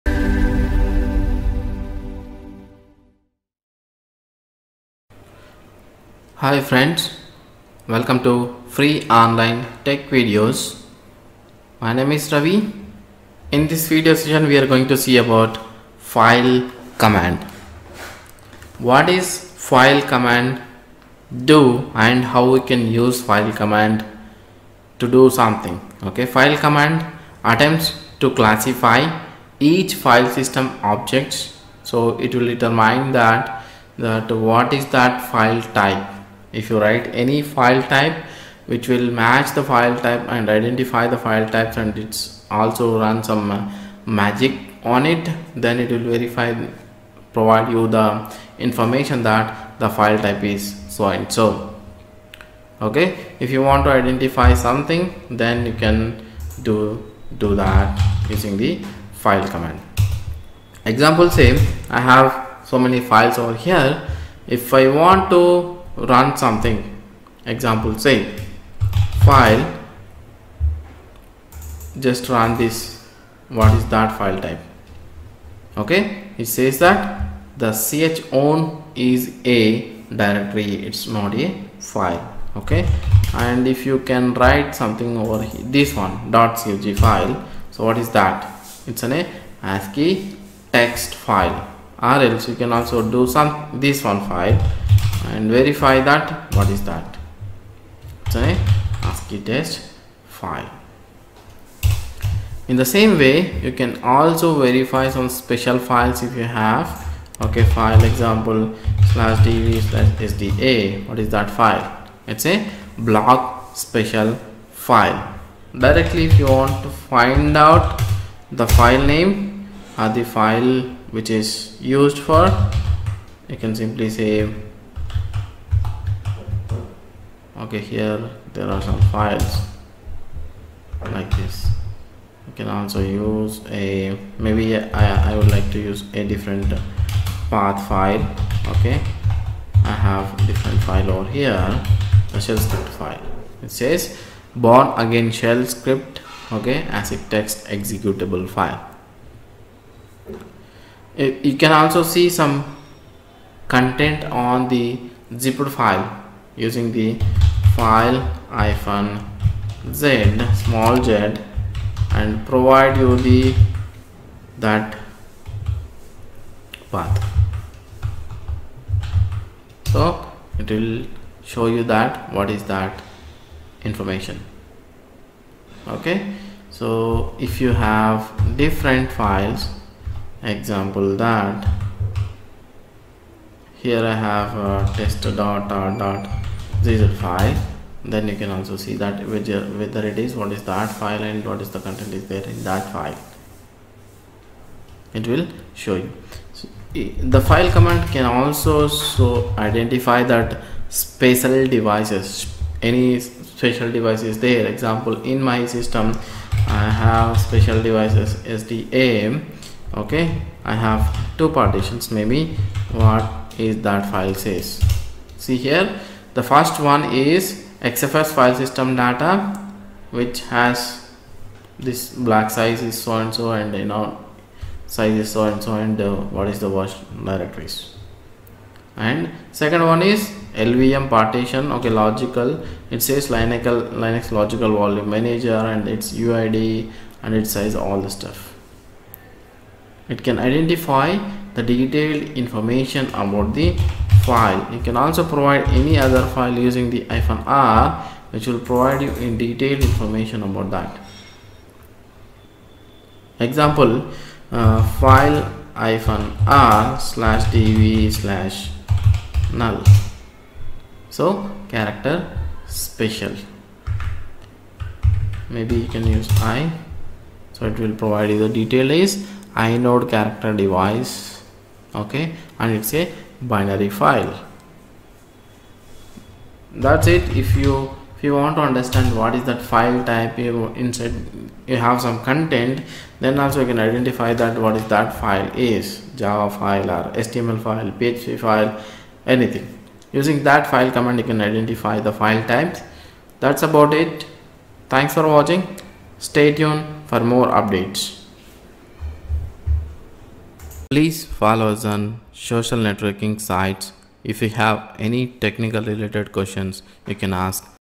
Linux Command Line Tutorial | file command to determine any file type | FOTV
file command to determine any file type